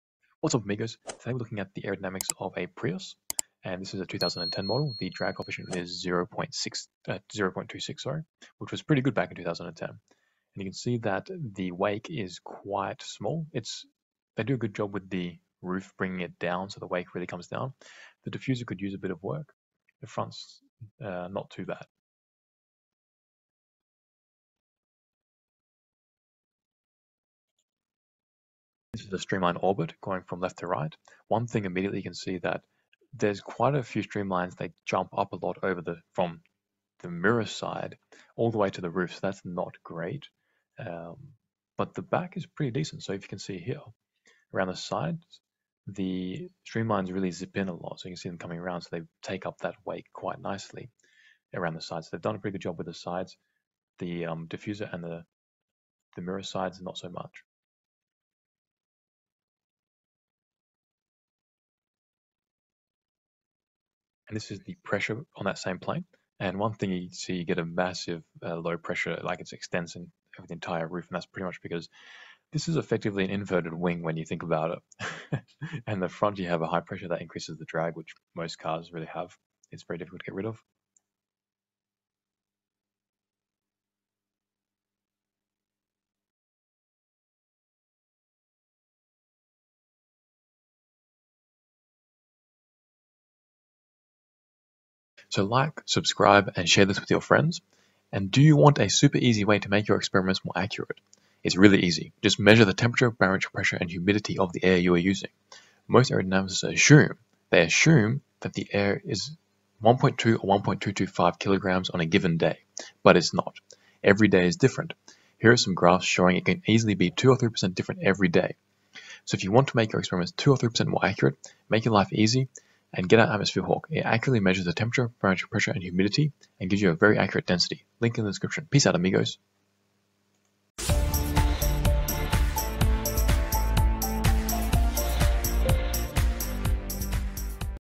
Prius Aerodynamics 2010 Model
The original electric car posterboy, the Toyota Prius 2010 model. What is its aerodynamics, drag coefficient and wake like?

Find out in this CFD simulation with OpenFOAM!

Check out The Atmosphere Hawk to make your aerodynamic experiments better